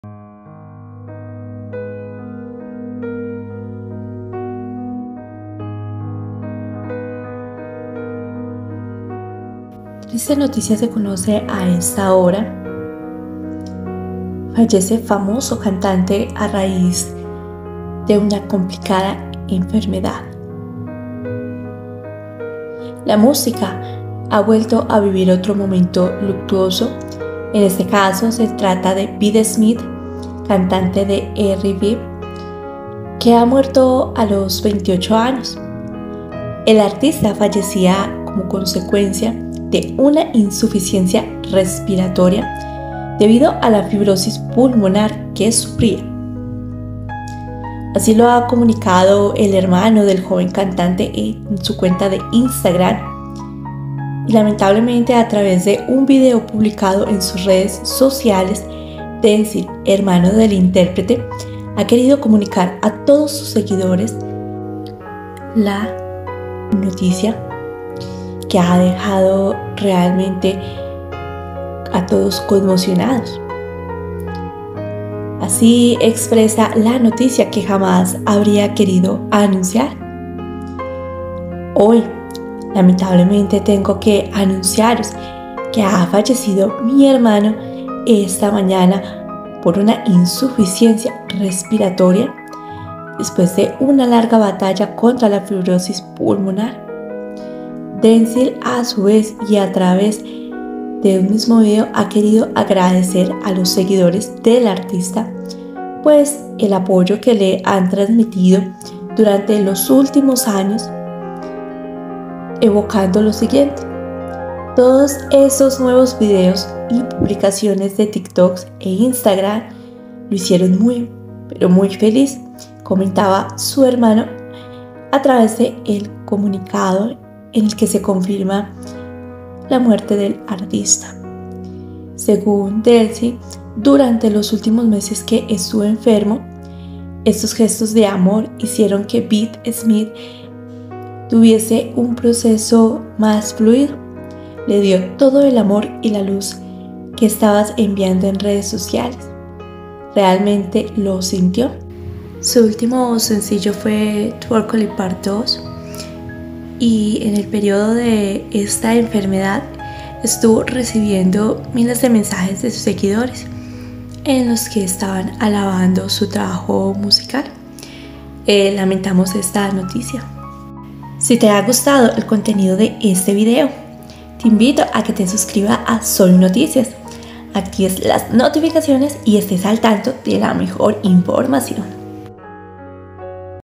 🖤🌹T3RR1-BLE PÉRDIDA para la música- Enfermedad terminal A..C..A..B..A con su vida.
@ soy noticias
TODO EL ESPETÁCULO CONMOVIDO A ESTA HORA POR  LA ESPERADA MU3R.TE DE FAMOSO ARTISTA. UNA FAT1-DICA ENFERMEDAD LO DEJÓ SIN VIDA.
🖤🌹T3RR1-BLE PÉRDIDA para la música- Enfermedad termina A..C..A..B..A con su vida.
!! BIENVENIDOS A @ soy noticias

Image

Ⓒ Video

Ⓒ Song ~

O interprete